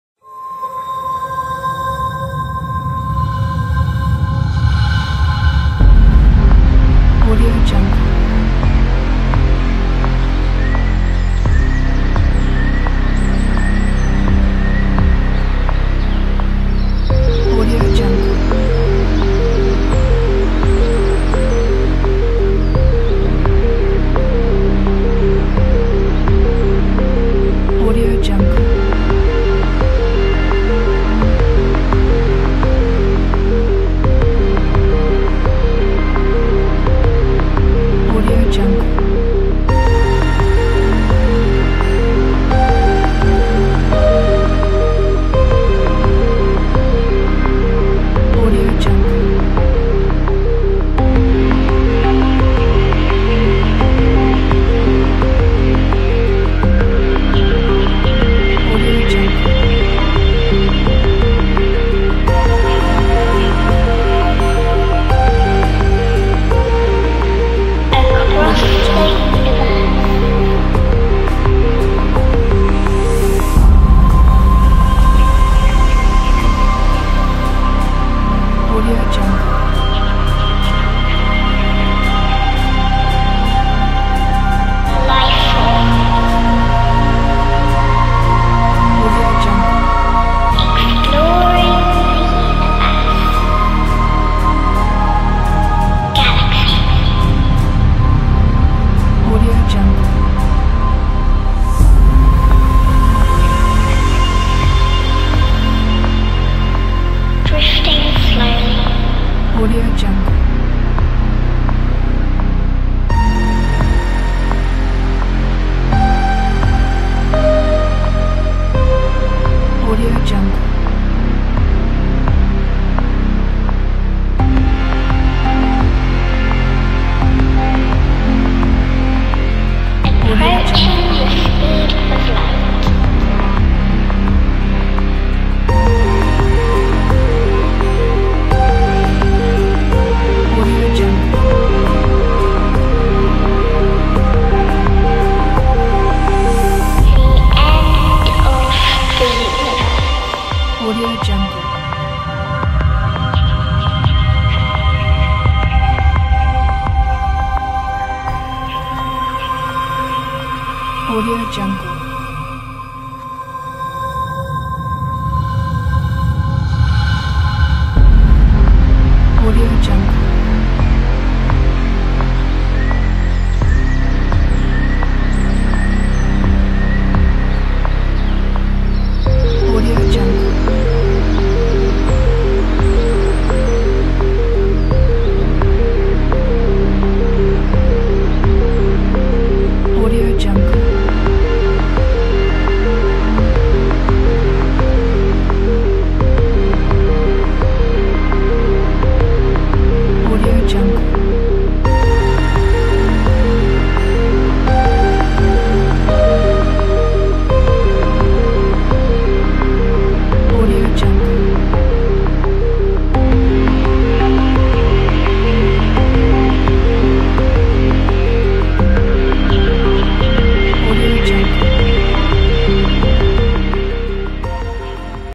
HUD UI Screens
Compatible with After Effects CC, CC 2015, CC 2018, CC 2019
Available 5 Screens
4K Resolution 3840×2160
Quick and Easy Customization
Plugins are not required
Music not included
Color Control
Font is Free